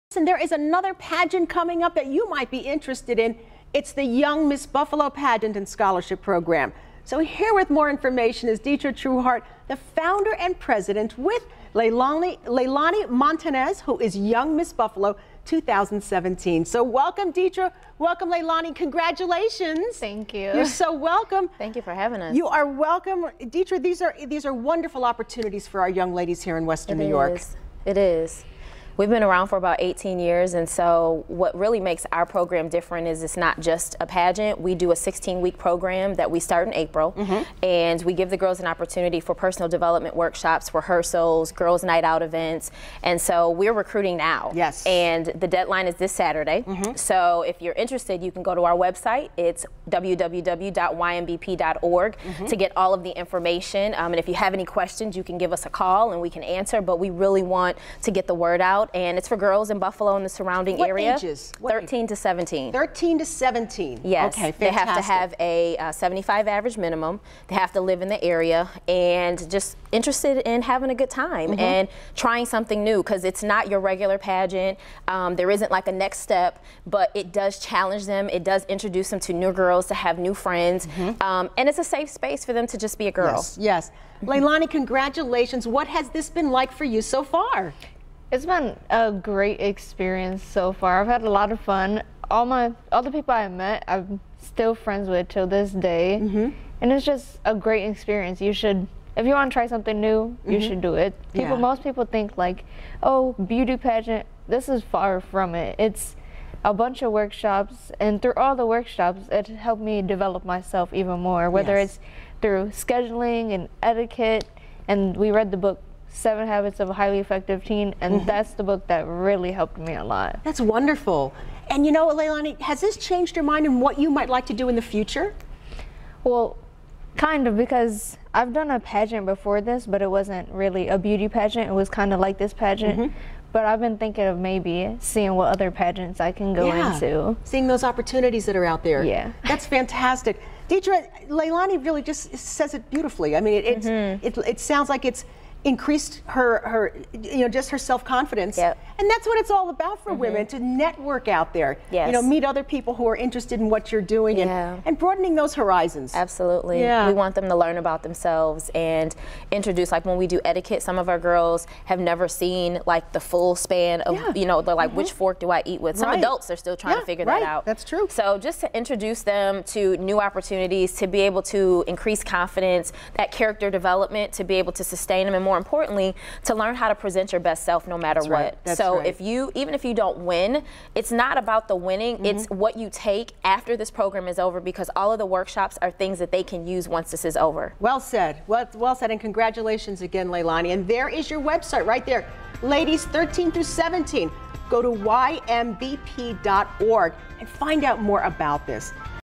Young Miss Buffalo Pageant and Scholarship Program
There is another pageant coming up that you might be interested in.  It's the Young Miss Buffalo Pageant and Scholarship Program.  Here with more information is Detra Trueheart, Founder &amp; President and Young Miss Buffalo 2017 Leilani Montanez.